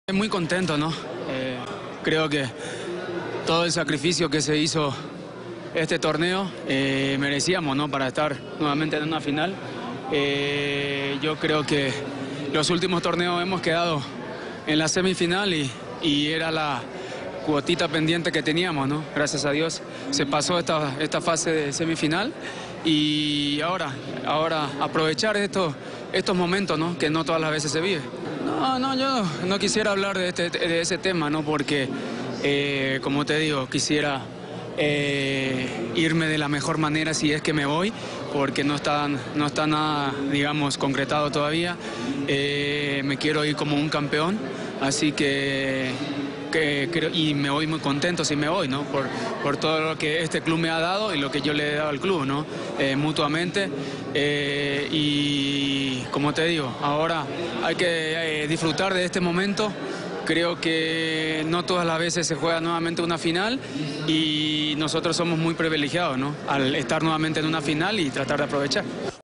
Osvaldo Martínez: “Si es que me voy, me quiero ir como un campeón”
El volante del América habló del significado de llegar a la final del Apertura y, sobre su futuro pase a Santos, dijo que “no está concretado, por ahora hay que disfrutar de este momento”.